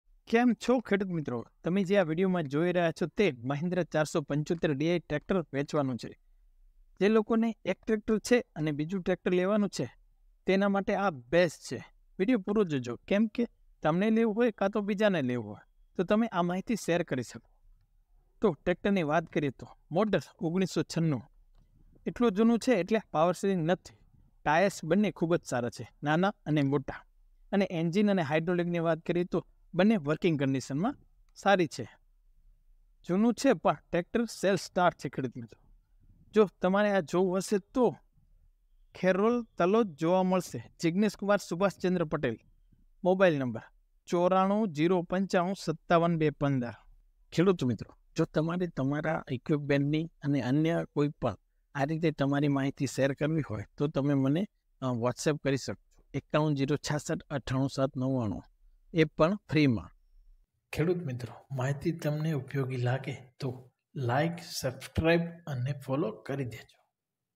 mahendra tractor second hand |મહિન્દ્રા 475 DI વેચવાનું છે. | Khedut Samachar Gujarat
મહિન્દ્રા.475 DI વેચવાનું છે.
મોડલ: 1996
આપવાની કિંમત 1,50,000
જીગ્નેશકુમાર સુભાષચંદ્ર પટેલ
ગામ ખેરોલ  તા તલોદ  જી સાબરકાંઠા.